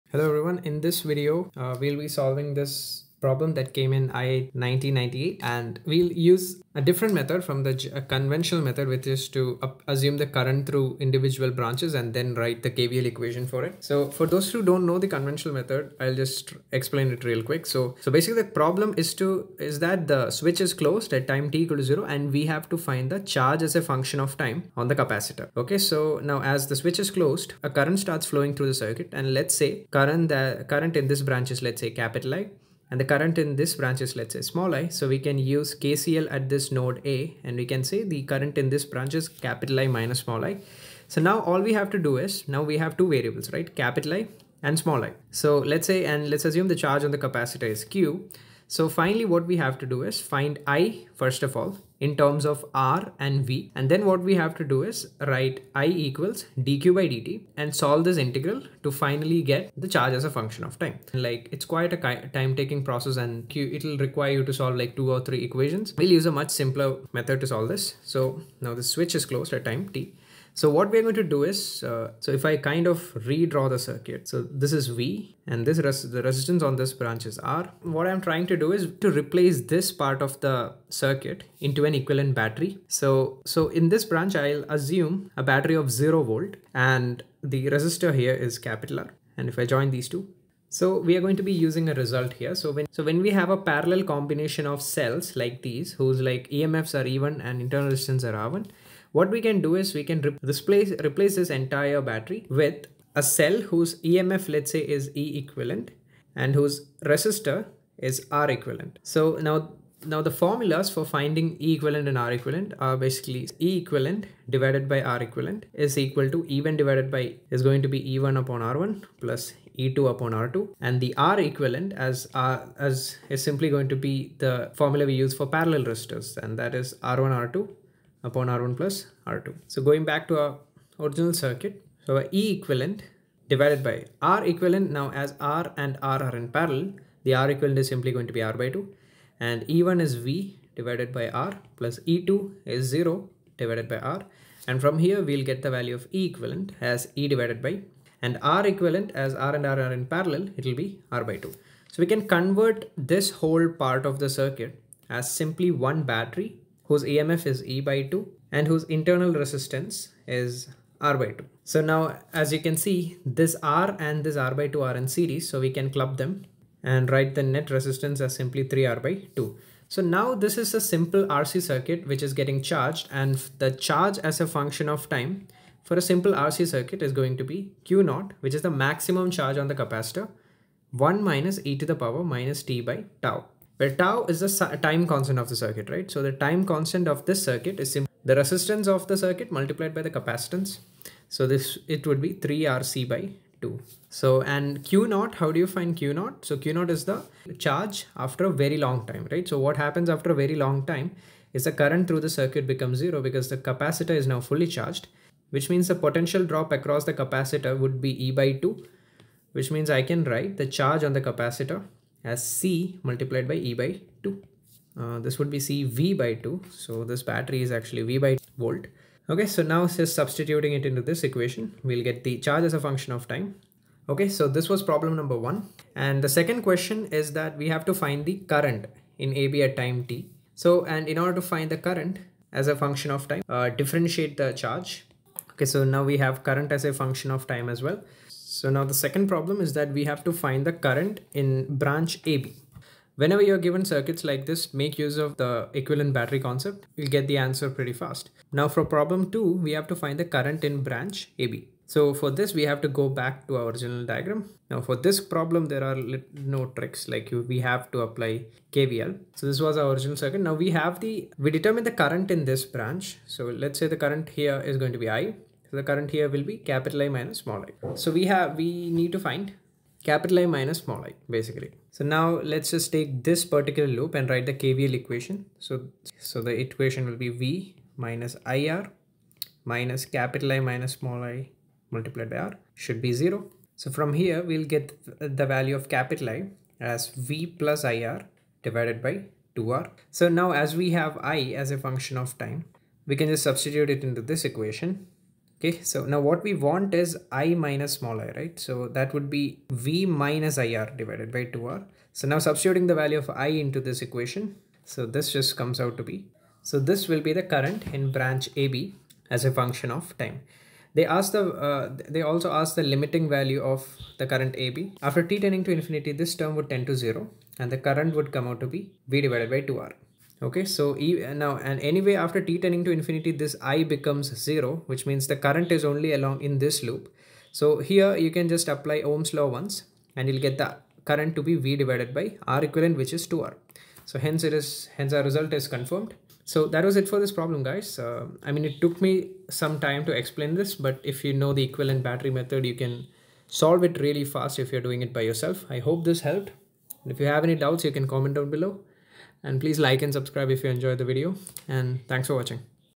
[IIT 1998] Q(t) of an RC circuit With Standard Results
Q : In the circuit shown in figure, the battery is an ideal one with emf V. The capacitor is initially unchanged. Switch S is closed at time t=0
 (a) the charge q on the capacitor at time t.
 (b) the current in AB at time t. What is its limiting value as t --} infinity

In this video i'll try to explain the shortcuts required to solve RC circuit problems in which we can exploit the equivalent battery concept.